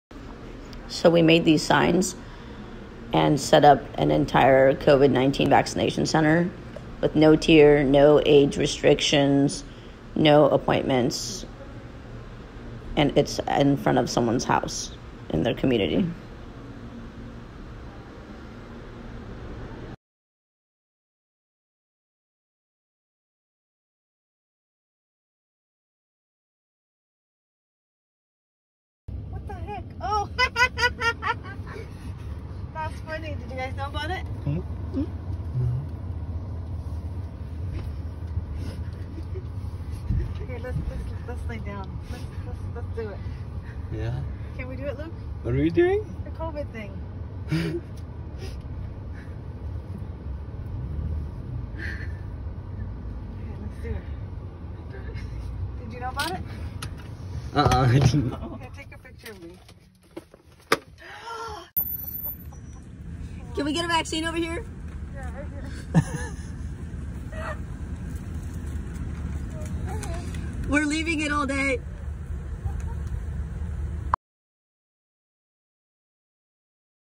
Community Vaccination Center Prank #11
I set up a fake vaccination center tent in front of someone's house